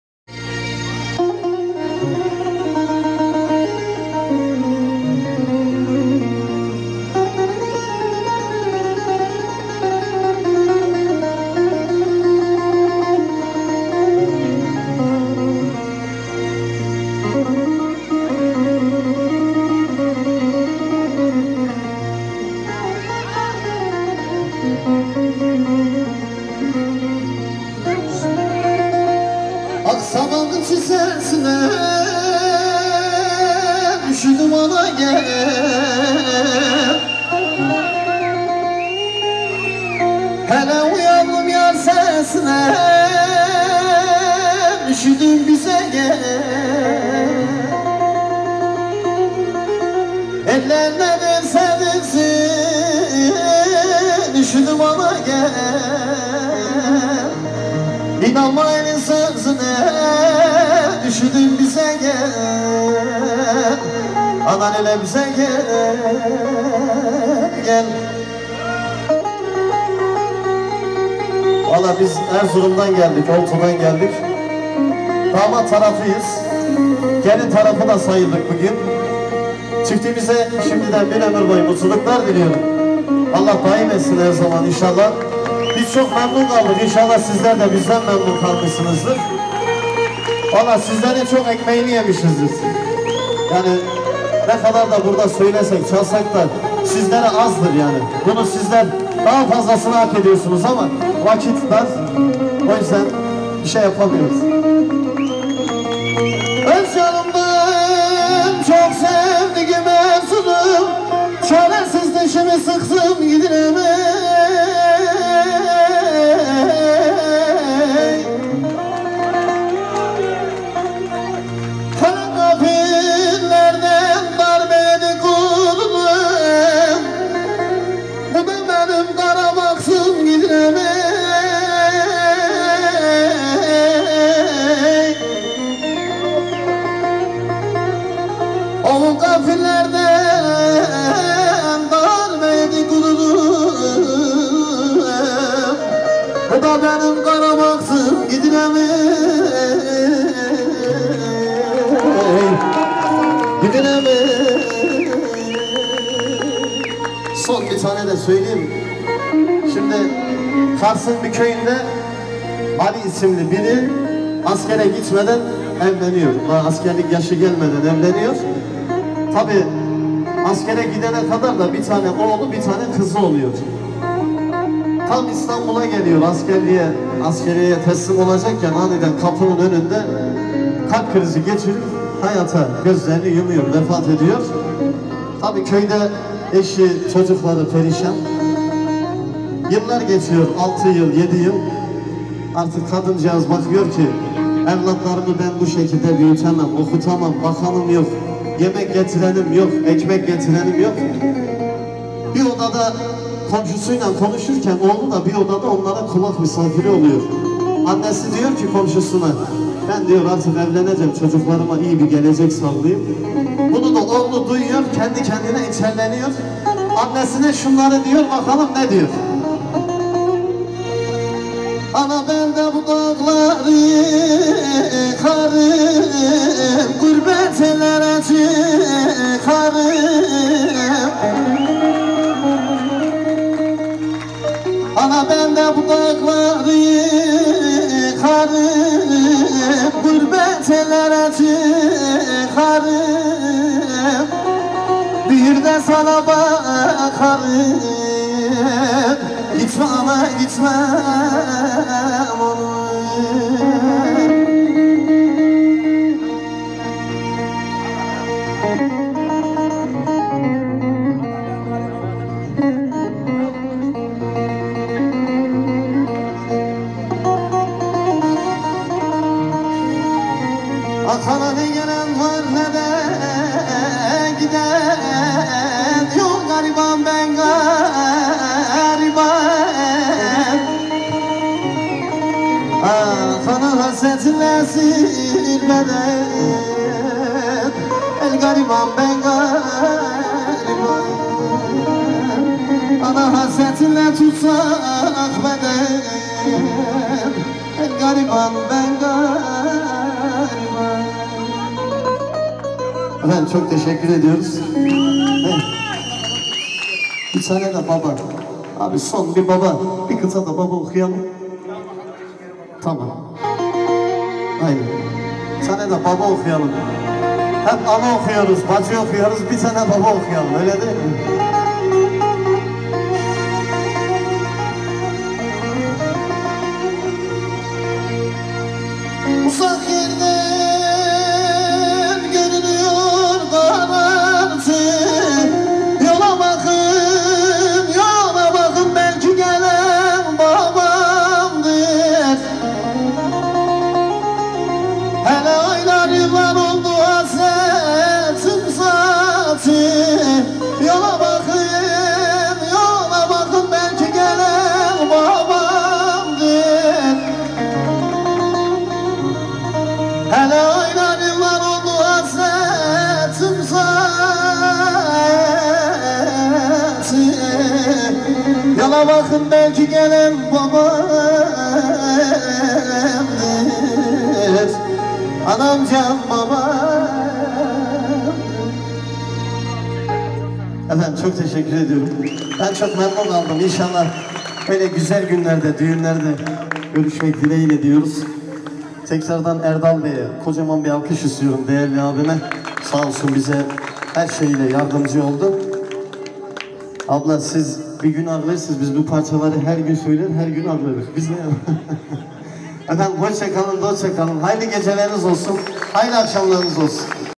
Mustafa özden uzun hava söylüyor.
Hasan Zerrin düğününde Mustafa Özden'den uzun hava